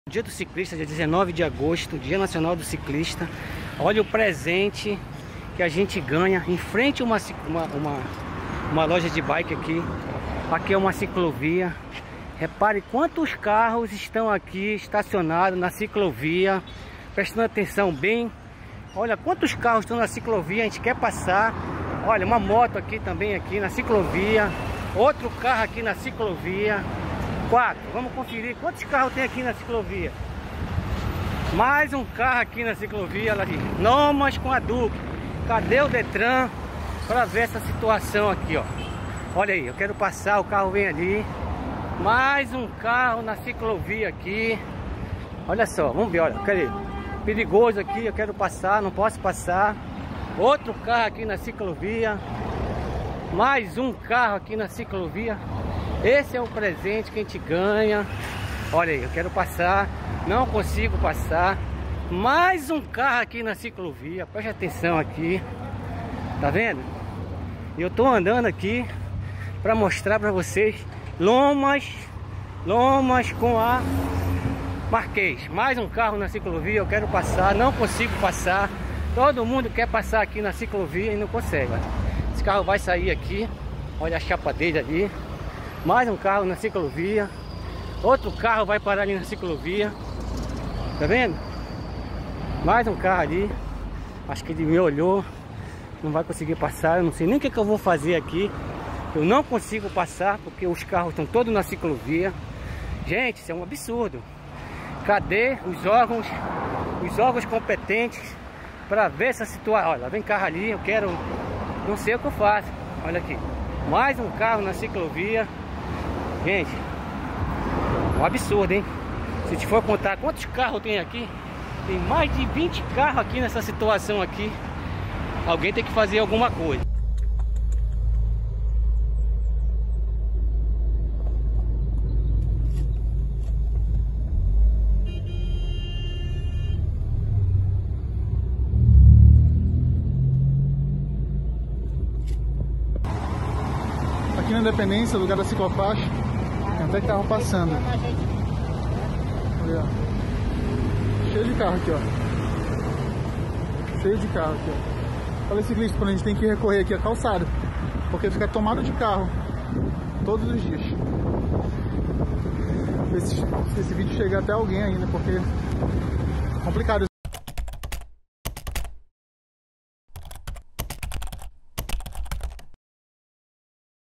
Internautas denunciam irregularidades no trânsito de Belém
Desrespeito aos ciclistas e pedestres é registrado em diferentes pontos da Região Metropolitana de Belém